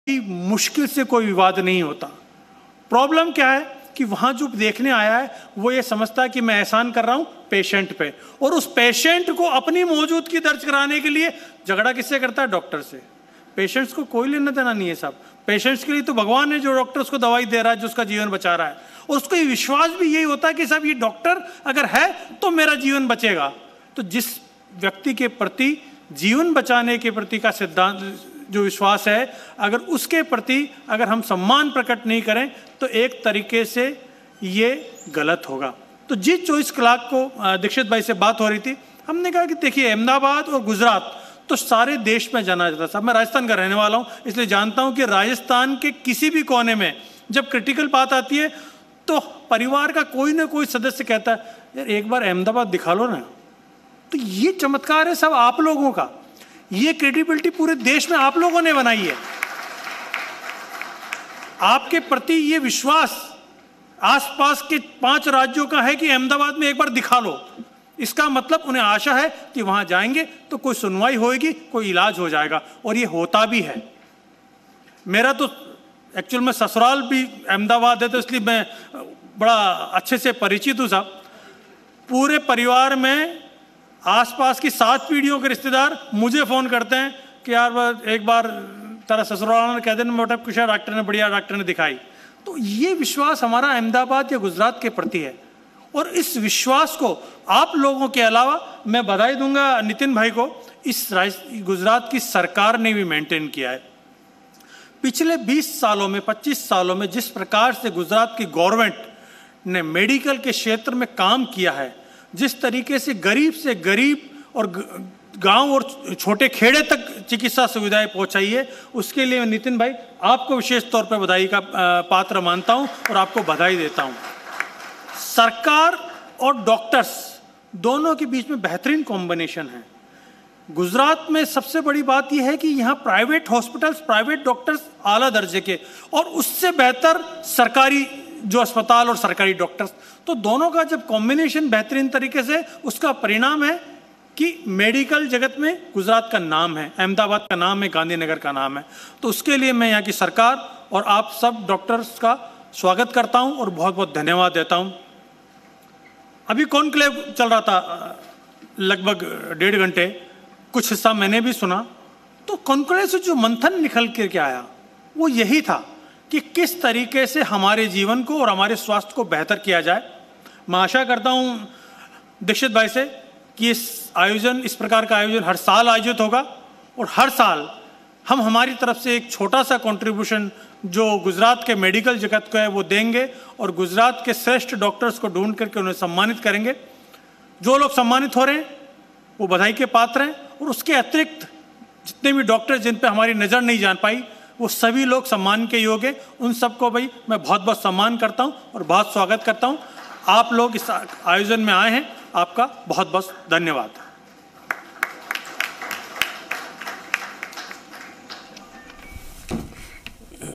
ZEEArogyaMahasanman2020 : चिकित्सा क्षेत्र में जी मीडिया की पहल
Gujarat : देश और विदेश में गौरव पाने वाले गणमान्य व्यक्तियों का बड़ा सम्मान, ZeeMedia के सीईओ एवं एडिटर इन चीफ पुरूषोत्तम वैष्णव ने कहा कि-'चिकित्सा क्षेत्र में जो लोग बेहतर काम कर रहे हैं उनको सम्मानित करना हमारा कर्तव्य है'।